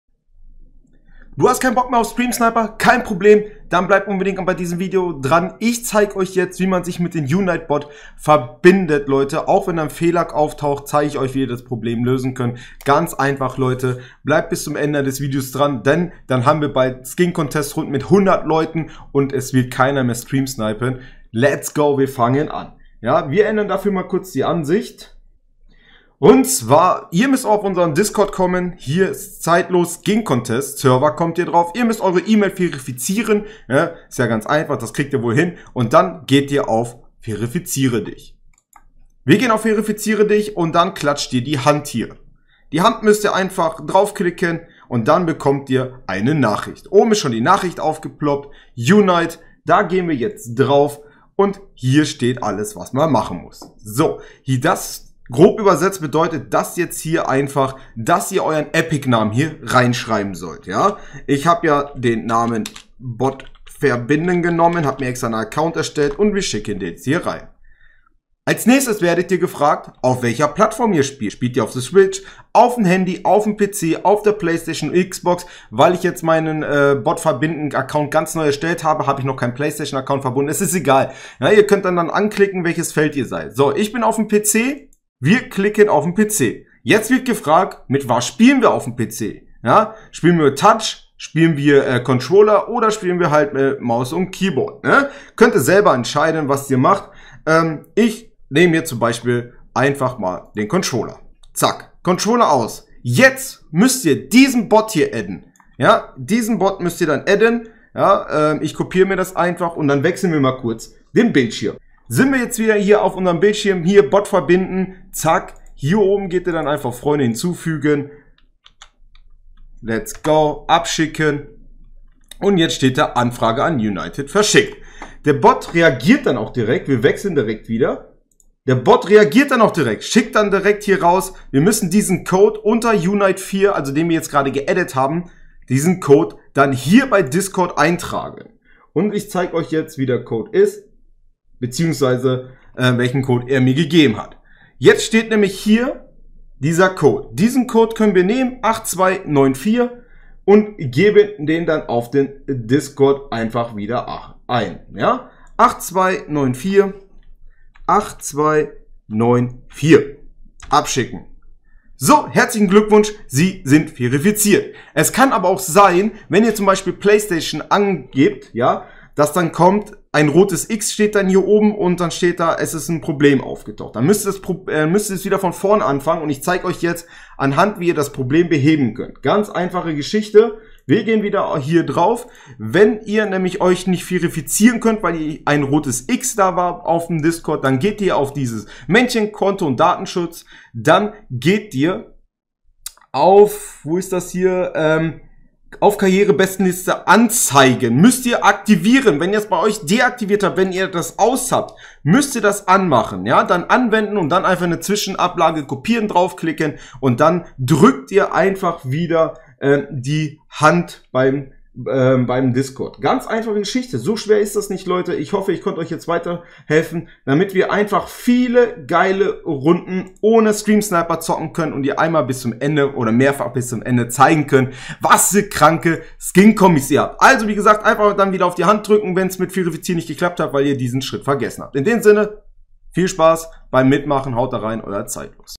Tipp| Skin Contest ohne Streamsniper | Yunite Bot mit Epic Account verifizieren. GER
Verifiziert euch und ihr werdet in Zukunft keine Streamsniper mehr in meinen Skincontest Runden, Custom Games oder Community Games mehr haben.
Gibt den Streamsniper keine Chance mehr !